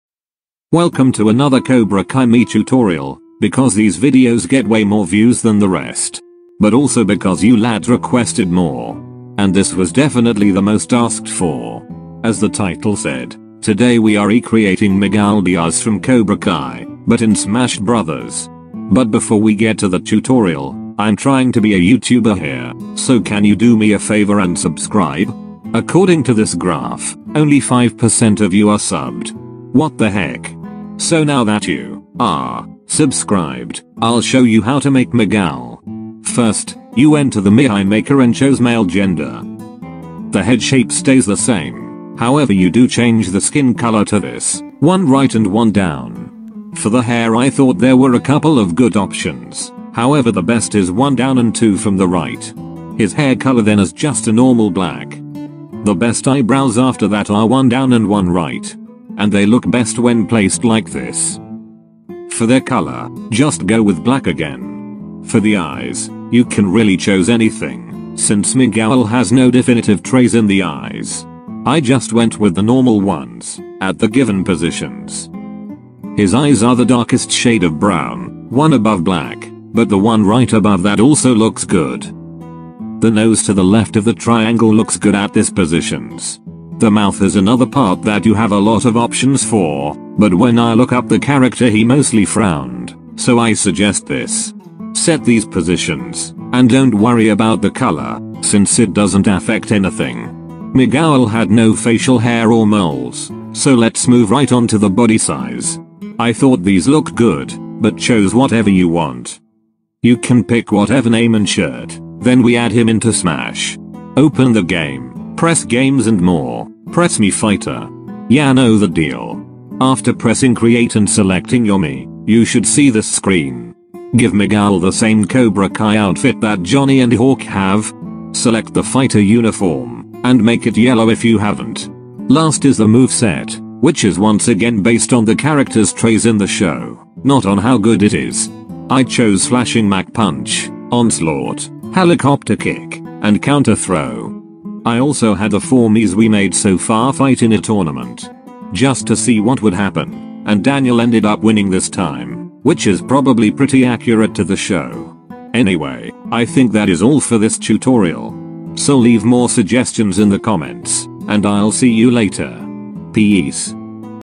Miguel Diaz | Cobra Kai Mii
Miguel Diaz from Cobra Kai as a Mii.

This tutorial was highly requested, so I hope it lives up to your expectations. I worked really hard on this video.    /(◕‿‿◕)/
As always, leave more suggesting in the comments. Mii tutorials or otherwise.

Remember: You are loved. I believe in you. Also Cobra Kai never dies.
Have a dapper day lads.